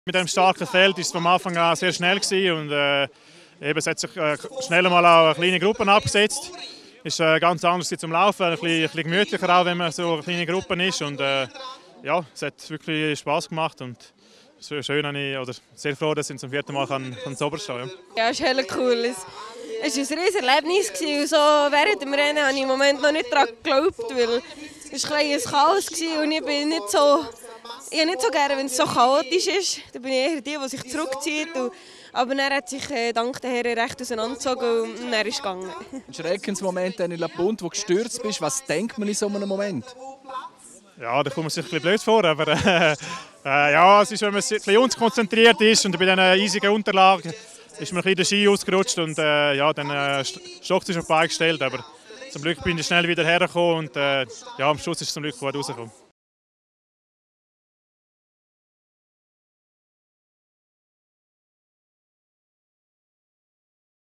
51. Engadin Skimarathon 2019: Die Sieger
Die Sieger des 51. Engadin Skimarathons Dario Cologna und Nathalie von Siebenthal  im Interview